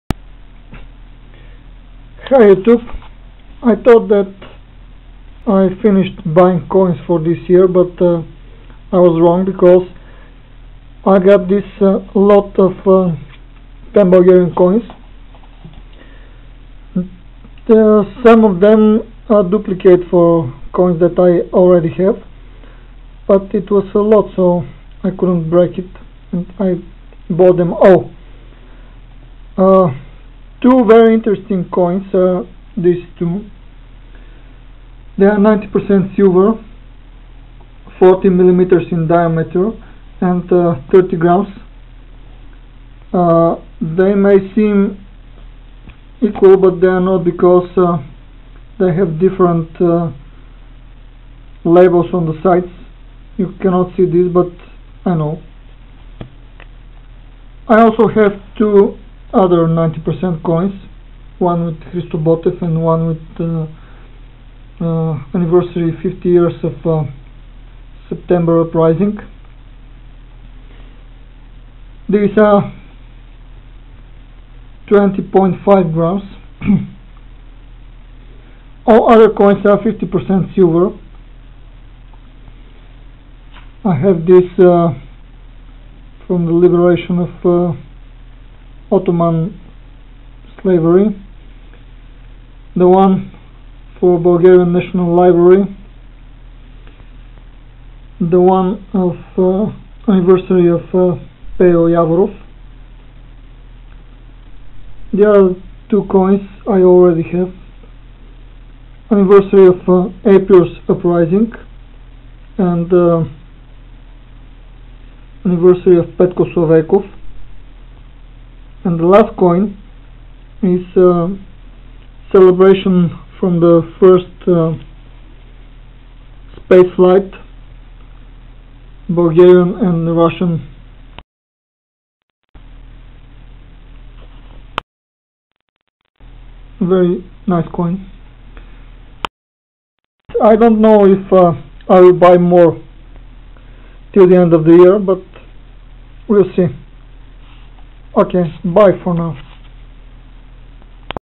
Stack update 131221
May be last silver for this 2013 year. 10 nice coins - four are 90% silver and six are 50% silver. All of them are Bulgarian coins. I'll make another video showing the interesting ones.
Sorry for the poor video quality, but I missed the good light.